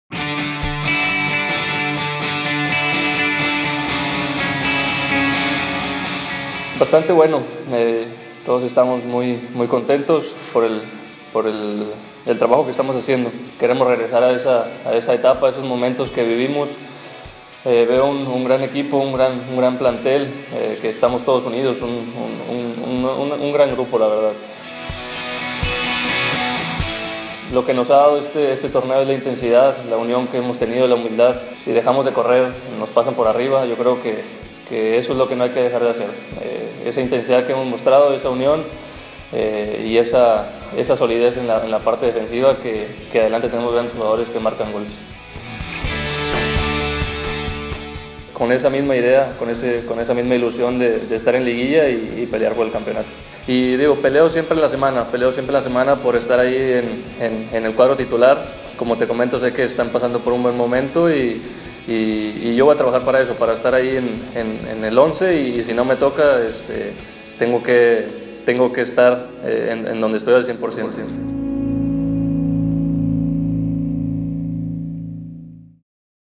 Hiram Mier (06/04/16)
Rueda de prensa con Hiram Mier del miércoles 06 de abril del 2016.